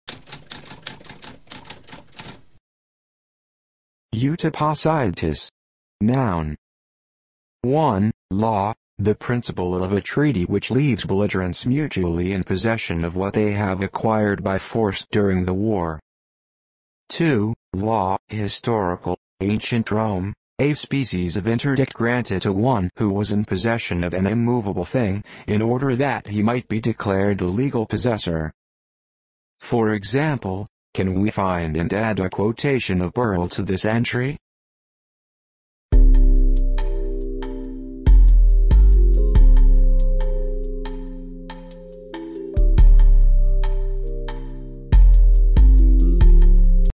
What does uti possidetis mean?
A spoken definition of uti possidetis.

Intro Sound:
Typewriter - Tamskp